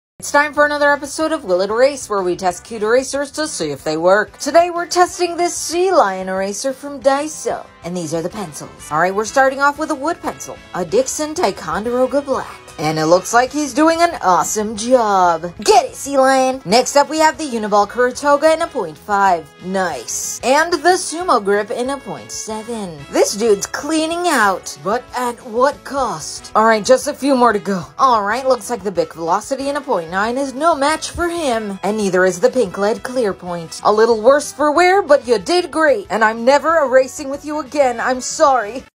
Will it Erase? Daiso SeaLion Eraser
Testing a cute Sea Lion Eraser from Daiso to see if it works.